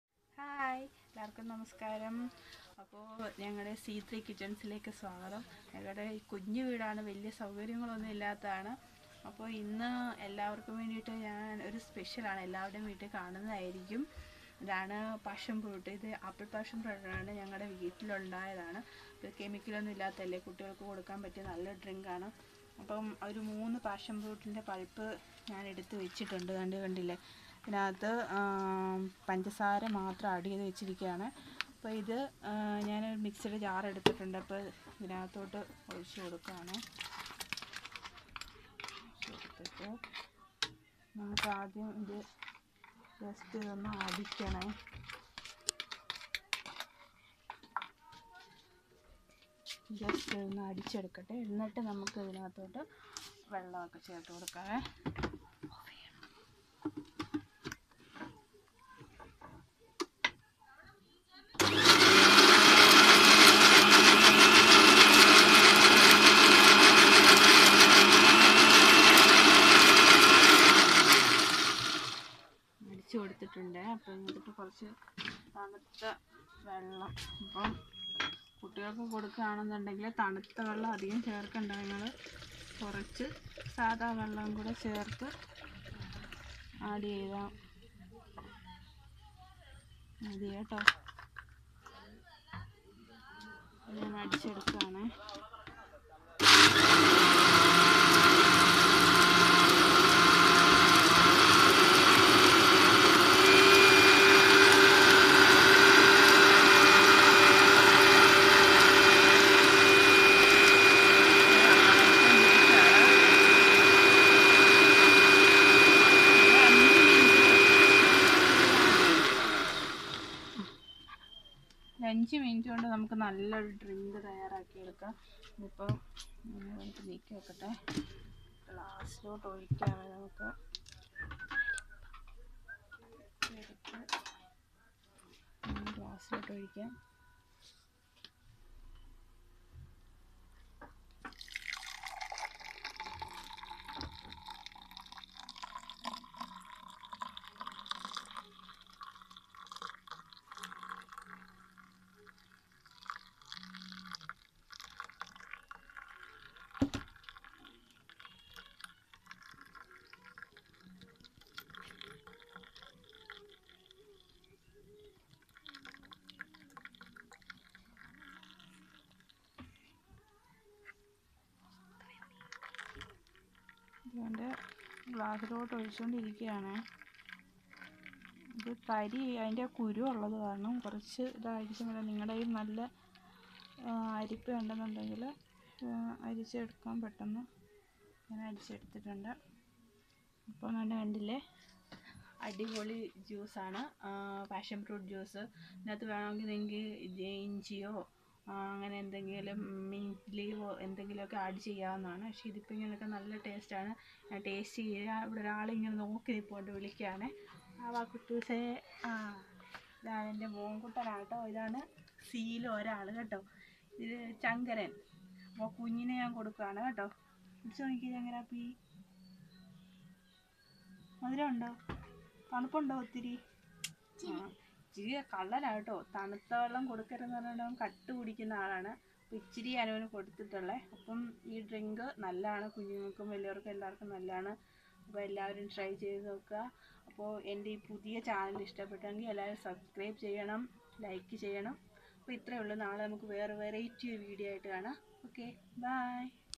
പാഷൻ ഫ്രൂട്ട് ജ്യൂസ് | Passion Fruit Juice | Sinu KG
ഈ ചൂടു സമയത്ത് വേഗത്തിൽ തയാറാക്കാൻ പറ്റിയ ഒരു ജ്യൂസ്